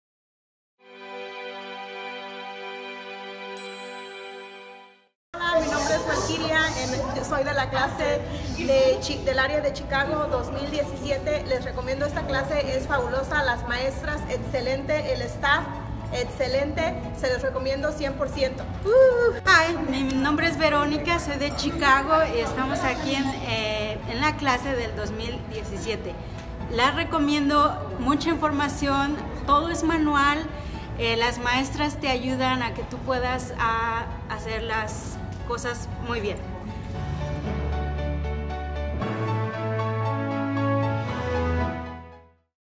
Hola amigos de IWED!
Aún tenemos lugares disponibles para nuestro curso de Diseñador de Eventos en español del 23-27 de Octubre 2017 en nuestro corporativo en Deerfield Beach, FL 33441

Va a estar genial!   Aprovecha esta oportunidad para llevar tu carrera al siguiente nivel  y certificarte.

Contáctanos, estamos para ayudarte